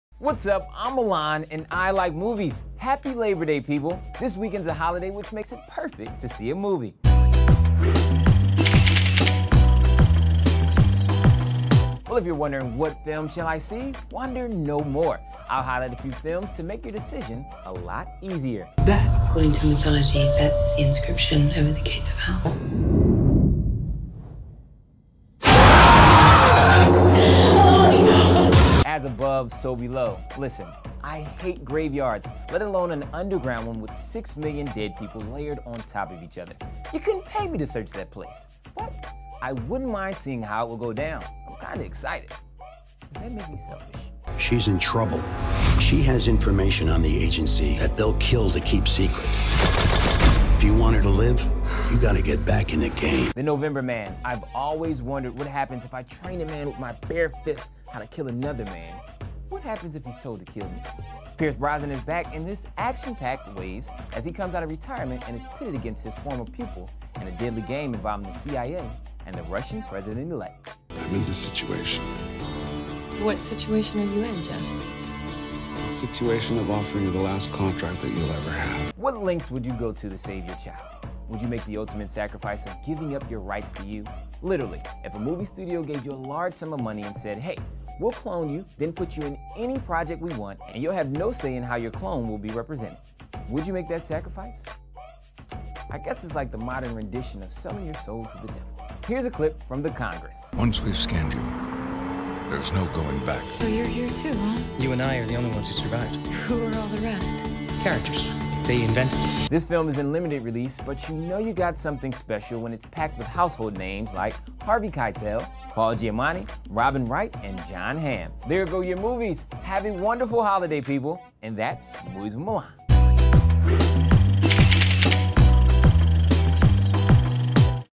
Labor Day Weekend Movie Guide: The Congress, As Below So Above & More
On this episode of Movies With Milan. Mr. Carter breaks down the must-see movies this Labor Day Weekend.

'Guardians Of The Galaxy' and 'Teenage Mutant Ninja Turtles' are still dominating the box office but, there's nothing like a great horror film or action-packed thriller to mix things up.

Check out the video above to see what films are hitting theaters this weekend.